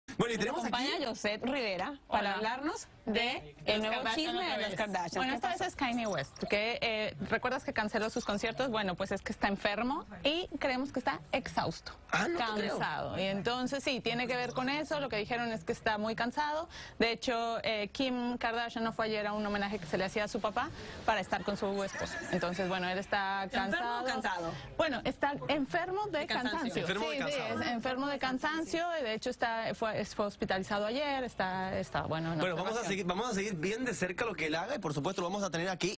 Kanye West está hospitalizado y tuvo que cancelar sus shows porque está enfermo de cansancio
El cantante tuvo que ser hospitalizado para recuperarse varias recaídas durante su gira musical. Su esposa Kim Kardashian permanece a su lado, pero aún no se ha emitido un comunicado oficial sobre el estado de salud de West.